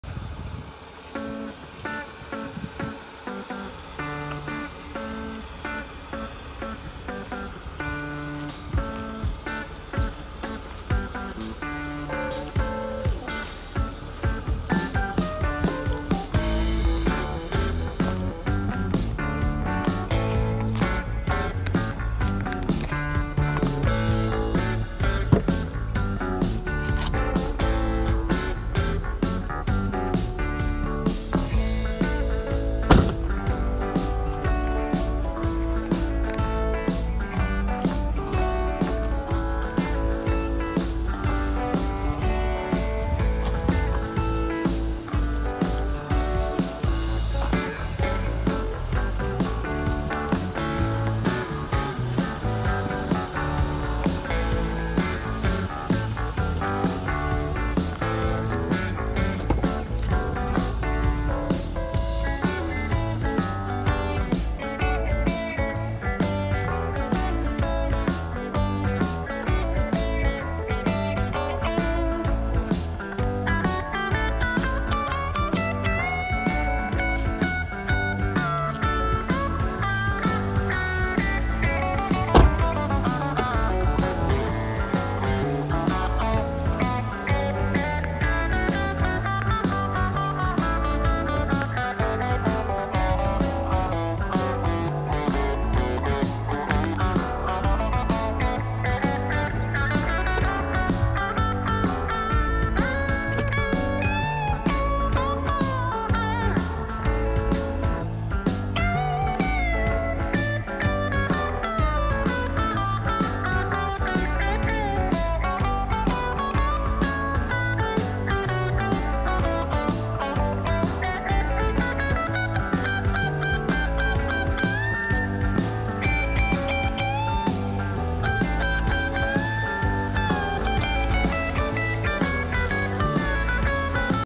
Virginia Cars - 2012 Jeep Grand Cherokee Laredo
(Stock # P0343)
This is a walk-around and walk-through of a Virginia Cars -  2012 Jeep Grand Cherokee Laredo with:
3.6L V6 Flex Fuel 24V VVT Engine
5-Speed Automatic
Leather
4WD